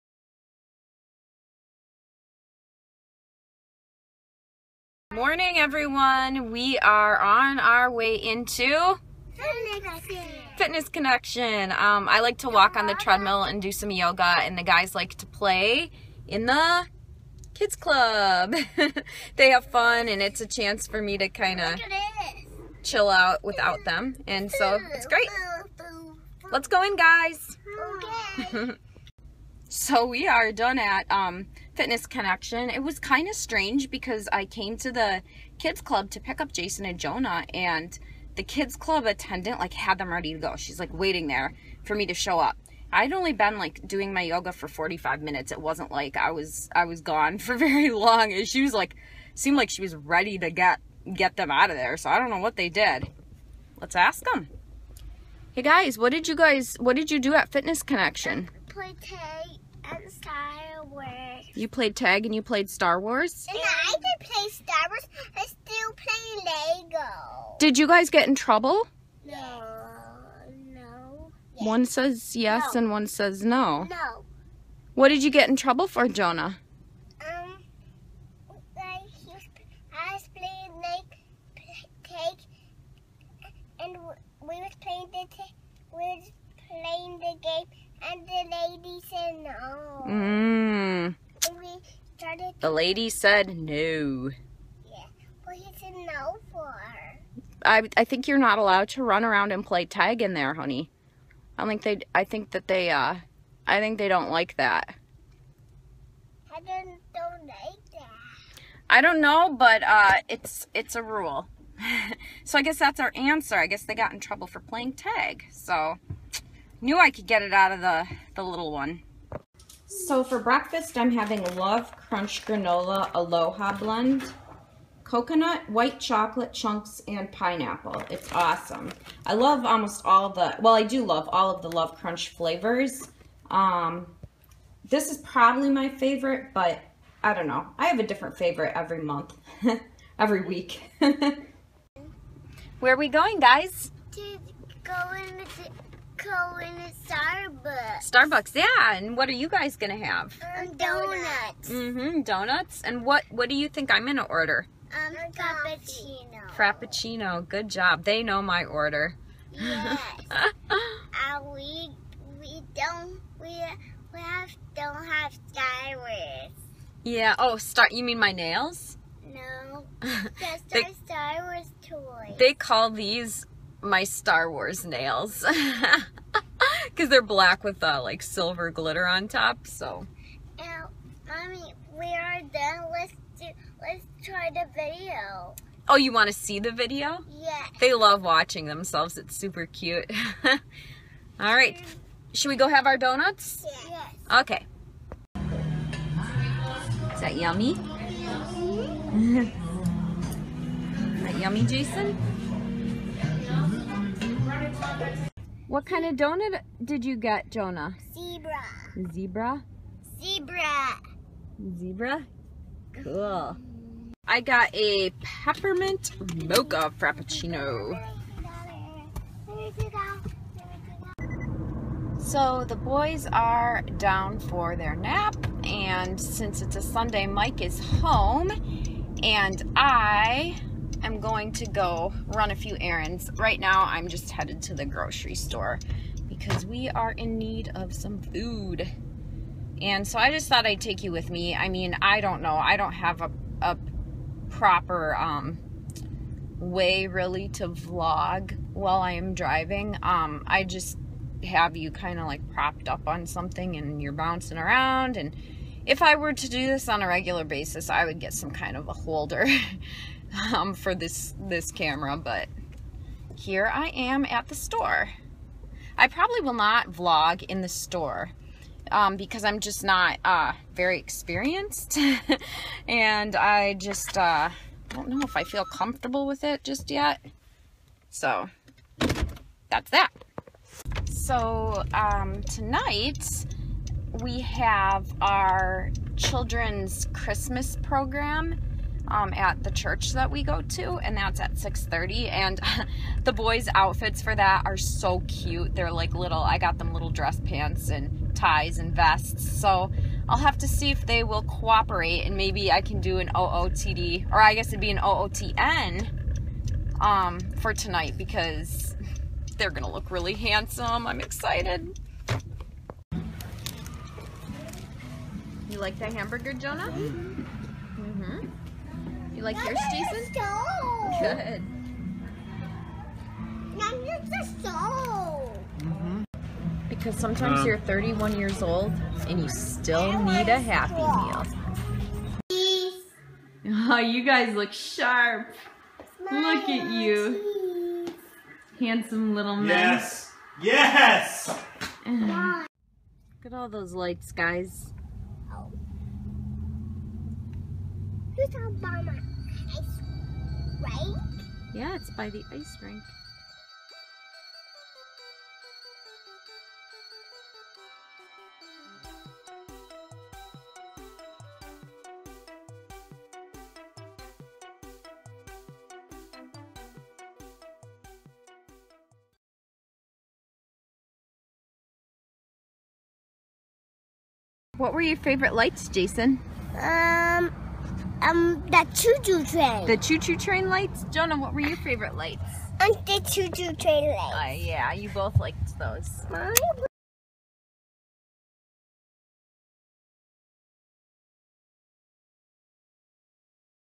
Family Vloggin' ~ Starbucks, Happy Meals, & Christmas Lights ~ 12/22/15
Just a little family footage I thought I would share! These are precious times and I want to remember them.